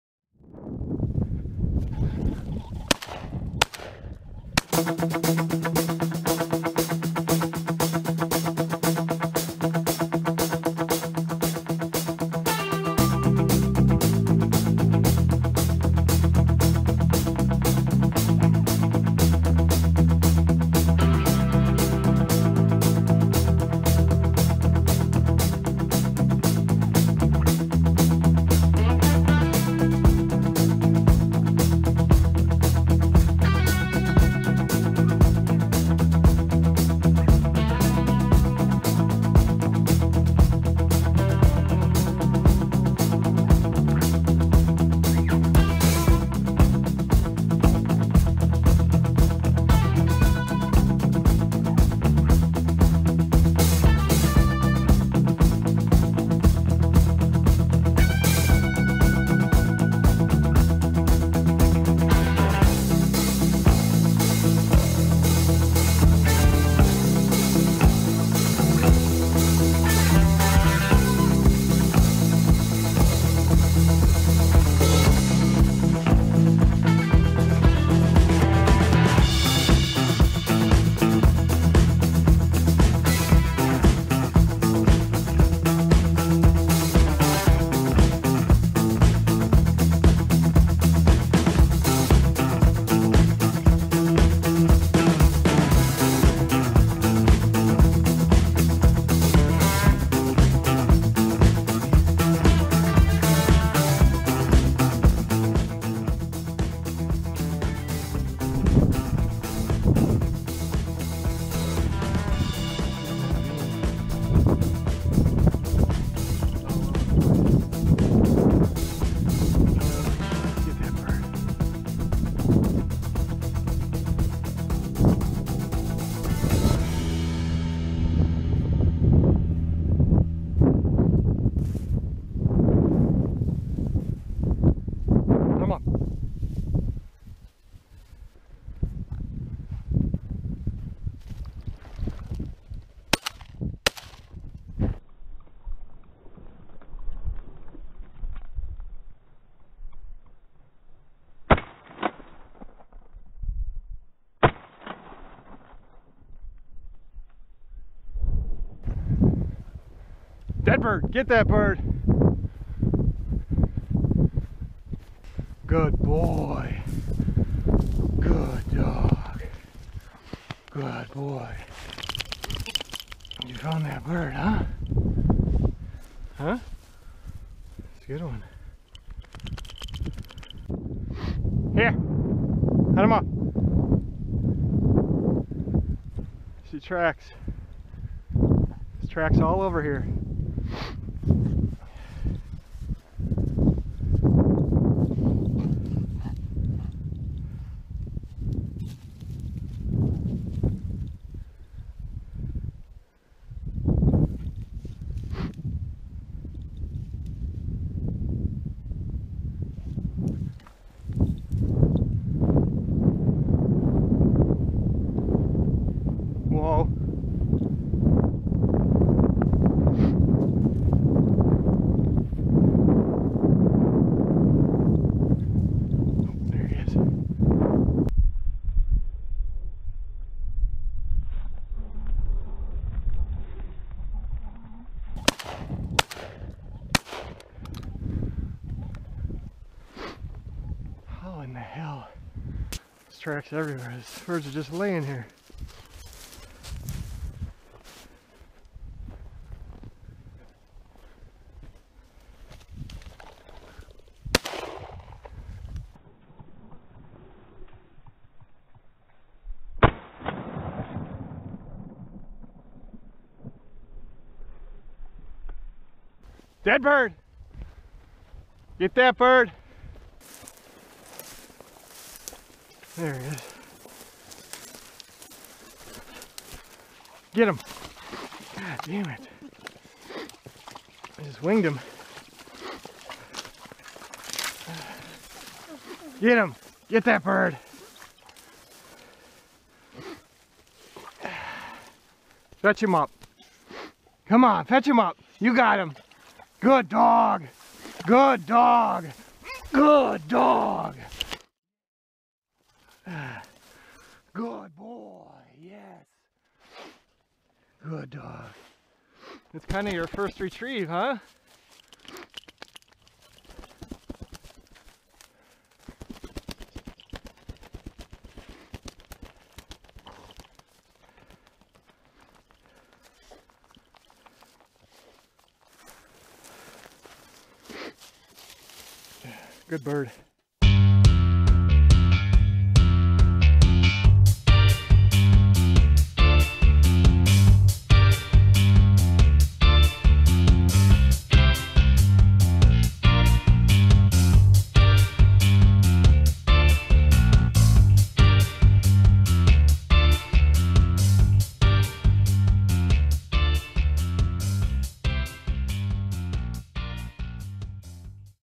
Pheasant Hunting Holiday
Hunting pheasants on public land in southern Wisconsin on Thanksgiving day.  It was cold but the snow was welcome in the field as the roosters left their path in it.  Later season forces the birds into ditch lines and fence rows (what's left of them that is).

In Wisconsin the daily limit is 2 birds and I couldn't convince anyone else to get out before the turkey meal that morning.  It was just me and the Vizsla again.  This being only his second full hunting season he's progressing nicely and he had his first retrieve out of a creek.  Ok, I get it, it was about a 5 foot retrieve, but that is progress!  The double rooster flush and shot surely means there was no shot in my loads, that was just plain awful.

Seeing 5 birds and shooting a limit is pretty good around here since this isn't SD and there are not the number of birds that other states have but the WIDNR has been doing a great job of creating habitat and stocking birds in certain areas.  I live in N. IL where there virtually isn't anywhere or anything to hunt on public land.